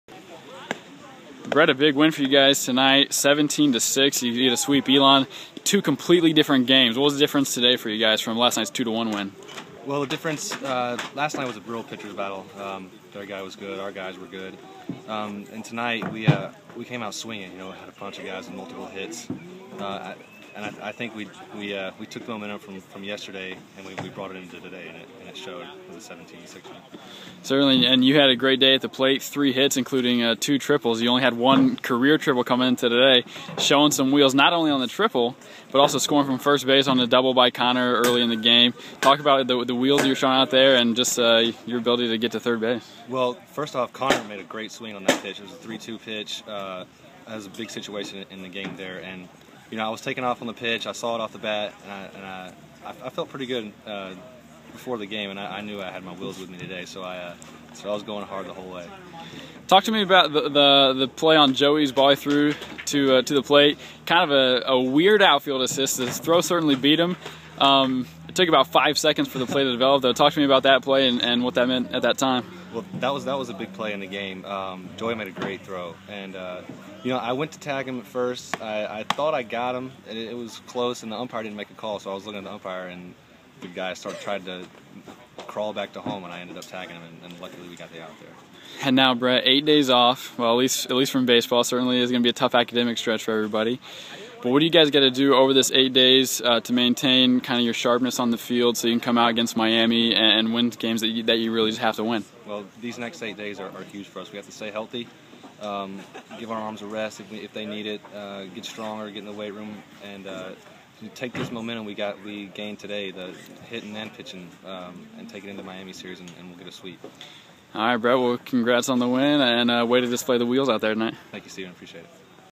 Brett Armour Postgame vs. Elon 5-1-13
Brett Armour talks about Wake Forest baseball's win over Elon on May 1, 2013.